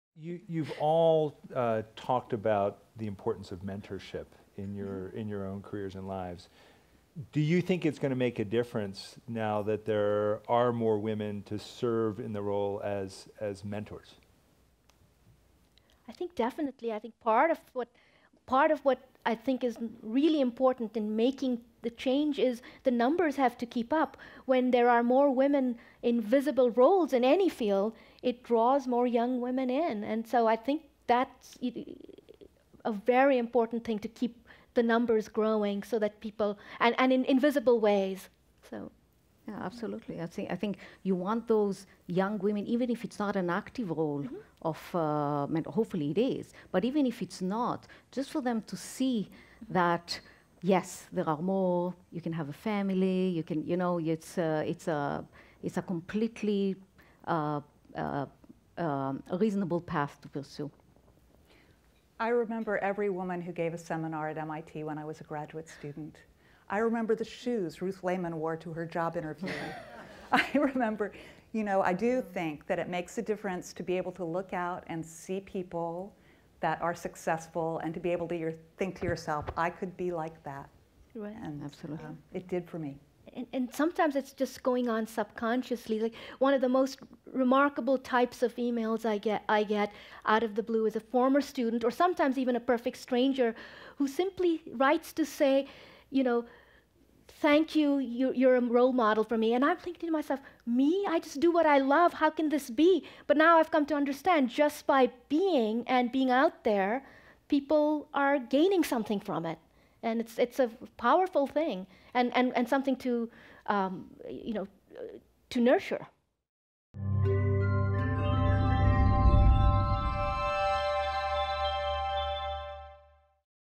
What is the role of mentorship in science?
At the 2016 Kavli Prize announcement breakfast during the 2016 World Science Festival, physicist Nergis Mavalvala, engineer Michal Lipson, and  2012 Kavli Prize Laureate in Neuroscience Cori Bargmann reflected on their mentorship experience and the influence that other female scientists had on their careers.